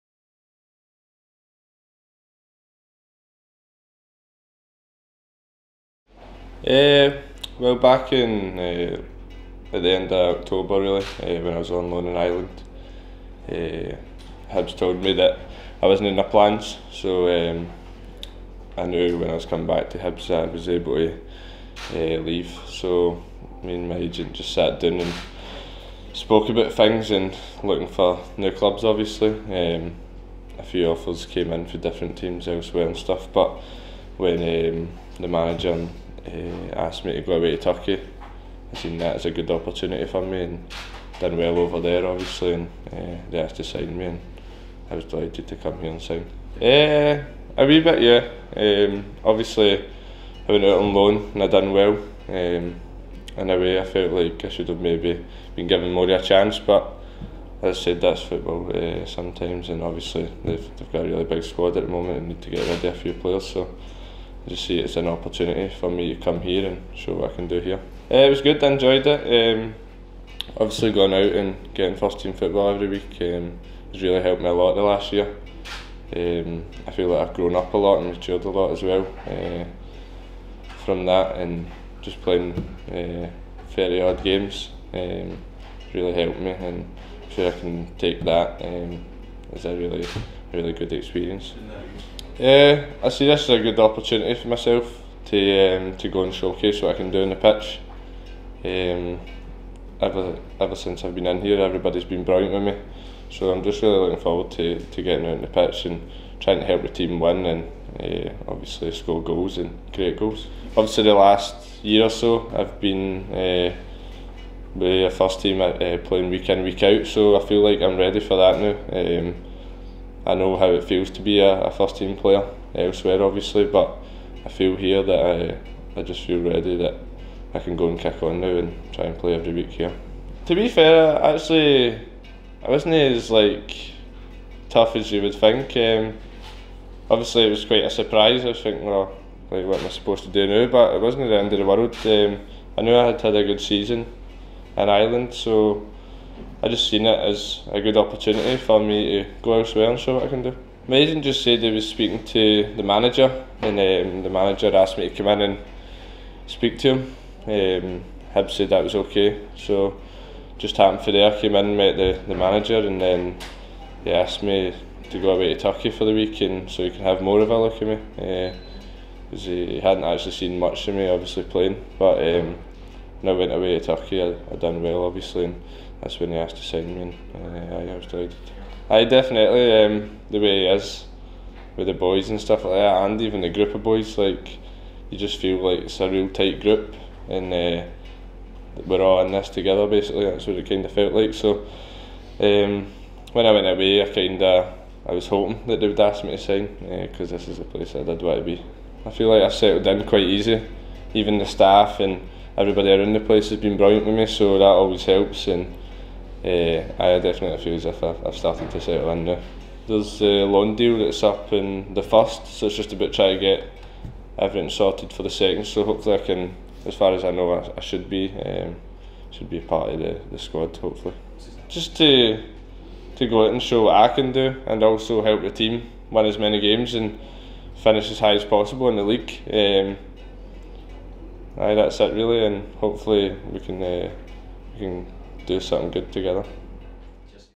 Steven Bradley looks ahead to Livingston's first match of 2023 against Motherwell
New signing Steven Bradley spoke to the press ahead of Livingston's home match with Motherwell in the cinch Premiership on Monday 2 January. Recently joining from Hibernian, Steven spoke through how his move to the Lions came about, his experience on loan in the League of Ireland with Dundalk this year, and his aspirations for the remainder of the season, starting with hosting Motherwell.